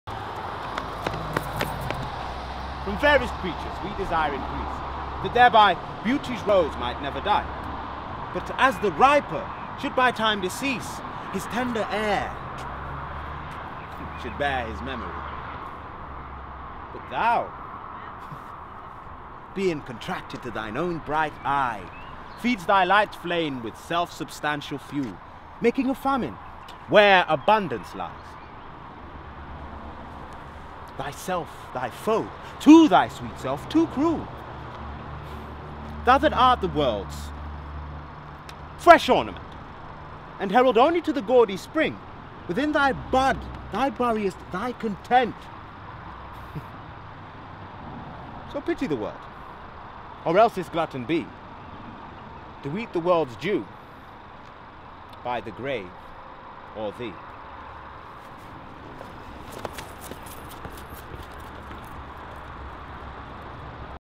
CHICKEN SHOP SHAKESPEARE - SONNET 1
Shakespeare's first sonnet was originally written as an encouragement to a young man not to waste his youth and beauty by remaining without child.  In our new piece we have re-contextualised it as good advice - from an older sibling to a younger brother - to have pride and confidence in his own personality, and emerging manhood.  The older brother is urging his growing counterpart not to throw away his potential -- and opportunities the world is offering him -- through a lack of self-belief.

Sonnet 1

Perfomed by: Lladel Bryant and Akwia Bryant

Producers: Tyron Maynard, Silas Parry, Lladel Bryant, Rob Blake